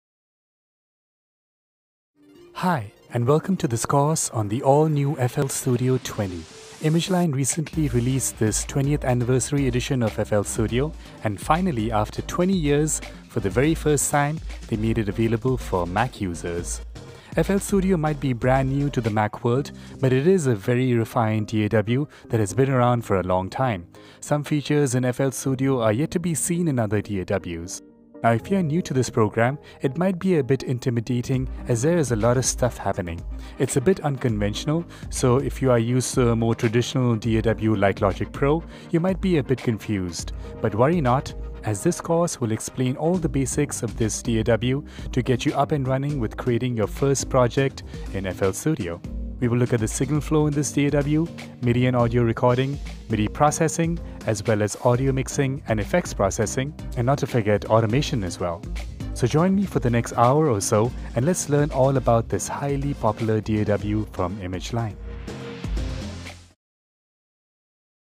FL Studio 101: Absolute Beginner's Guide - 1. Introduction
FL Studio 101: Absolute Beginner's Guide by Rishabh Rajan
Video 1 of 21 for FL Studio 101: Absolute Beginner's Guide

From its early beginning as FruityLoops, FL Studio has evolved into a very advanced music production system  which has a very different workflow from other DAWs. Used by renown artists such as BT, Afrojack, Mike Oldfield, the late Avicii, and many more, you too can join the FL Studio party by watching this course by trainer and producer Rishabh Rajan.

First, you learn about the different versions of FL Studio to help you choose the right package for your needs.  You learn how to get started quickly with templates, how to navigate the user interface, and how to find samples, plugins and presets in the browser. You discover FL Studio's unique approach to step sequencing, and you learn how to record and edit MIDI and audio. And once all your parts are recorded, you learn to arrange, automate, mix and export your final project.

Regardless of whether you're on a Mac or a PC, join trainer and music producer Rishabh Rajan in this course and learn how to get started the right way with FL Studio 20!